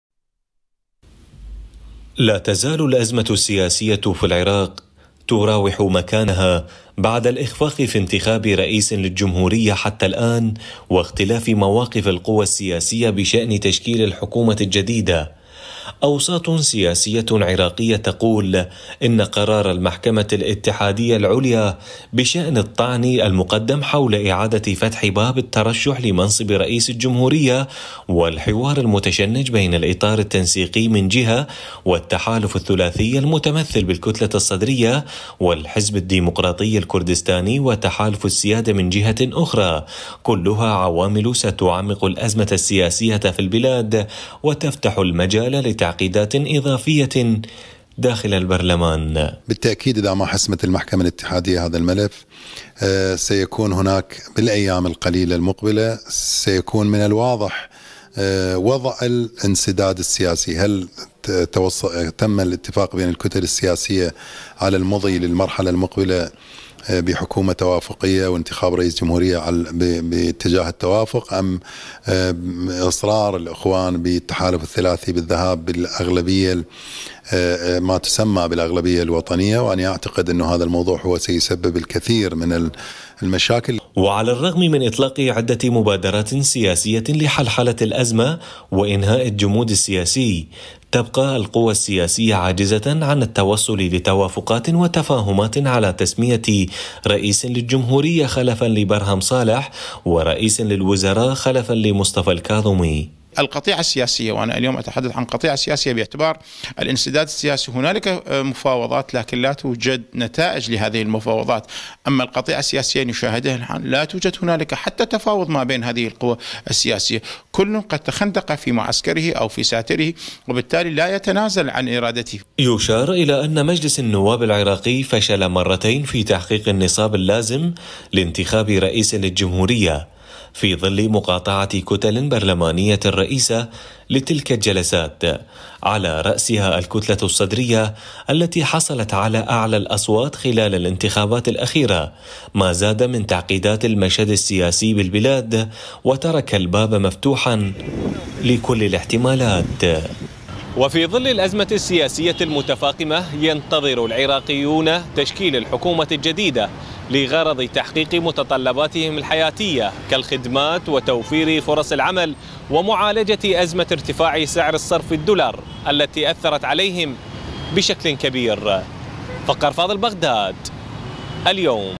العراق.. تحذيرات من عرقلة اختيار رئيس الجمهورية وتشكيل حكومة جديدة
إعداد - فقار فاضل

تابعوا قناة اليوم على التردد:
 12687_27500 (H) _NILESAT